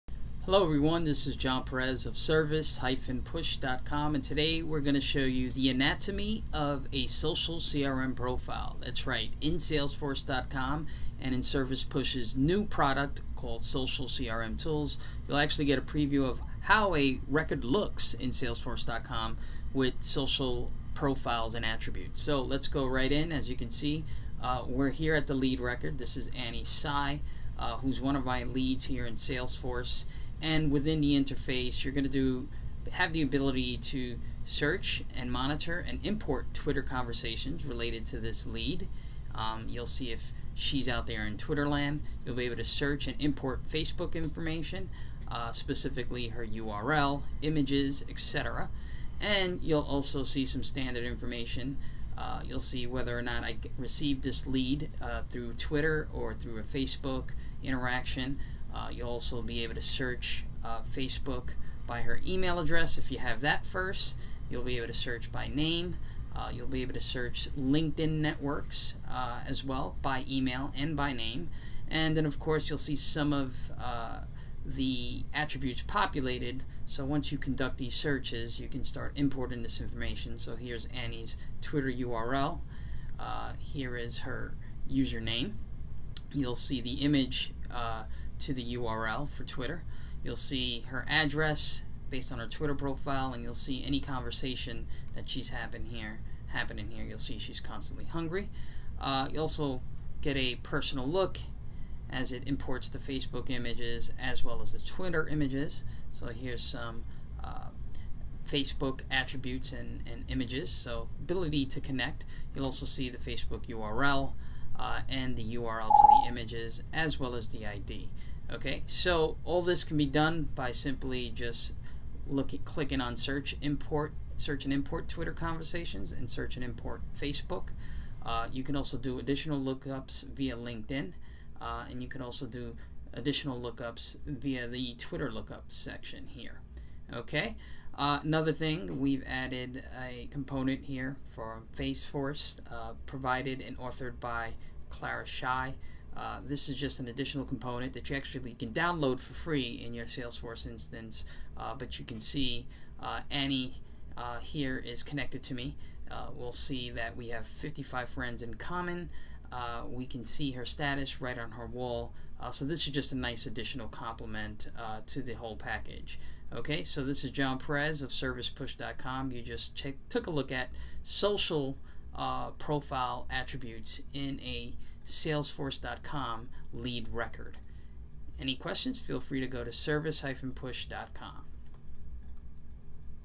Anatomy of a Social CRM Profile in Salesforce.Com
Service-Push's new suite of Social CRM Products for Salesforce.Com illustrates how to link Twitter, LinkedIn, and Facebook information about your customer right within Salesforce.Com.  Attached are a high level suite of features:

Lookup Twitter, Linkedin, and Facebook information regarding your Contacts.
Automatically Monitor Customer Conversations.
Seamlessly Create Leads from Twitter
Import Twitter Conversations into Lead or Contact Records.
Import Facebook Profile information into Lead or Contact Records.
Track Lead Source Information as a Facebook, Twitter, or Linkedin Source.
Track company conversations.
Lookup Company Fan Pages.